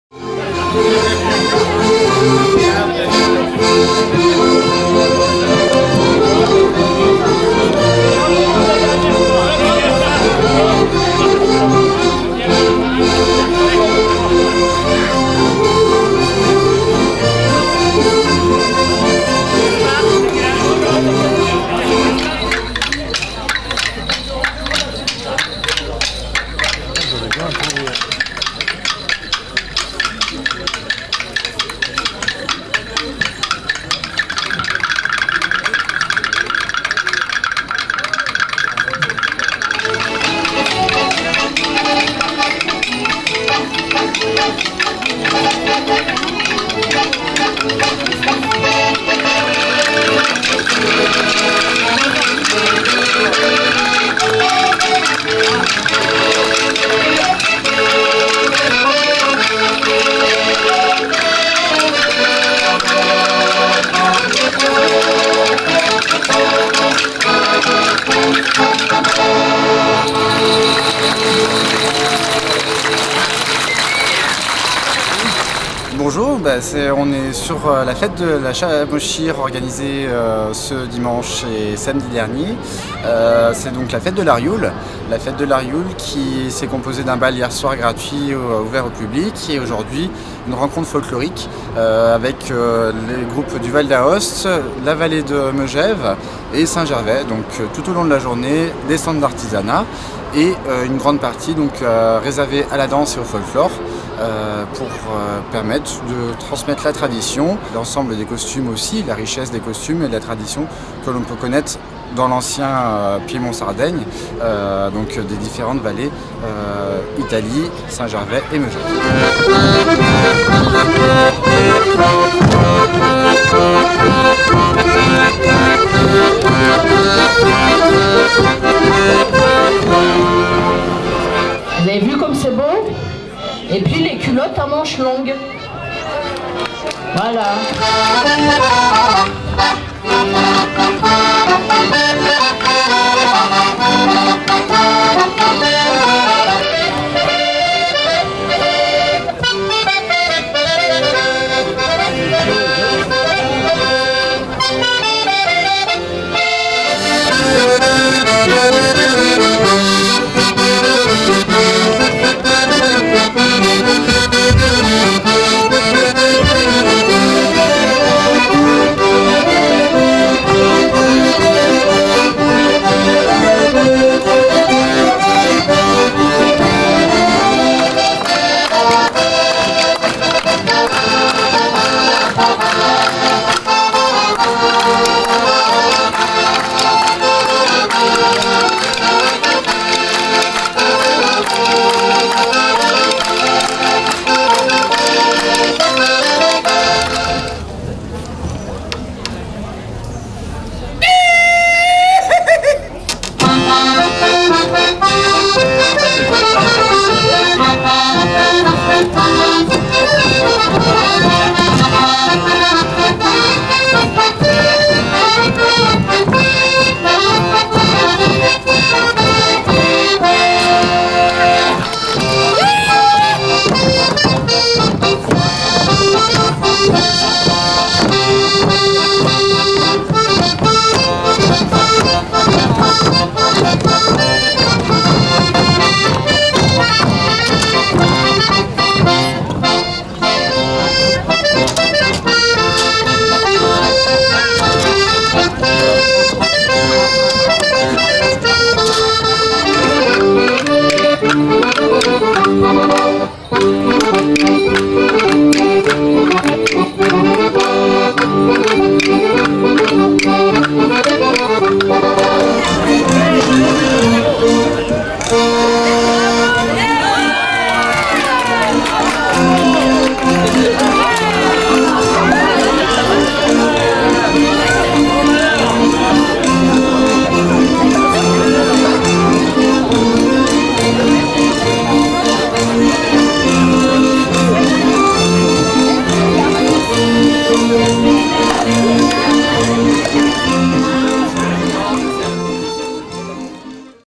LA RIOULE Saint-Gervais Mont-Blanc spectacle de groupes folkloriques culture montagne tradition
Août 2017, culture montagne... LA RIOULE, spectacle de groupes folkloriques... La Clicca d'Aoste, Les Mailles et béguines de Megève, La Chamoschire de Saint Gervais Mont-Blanc... Danse et musique traditionnelles...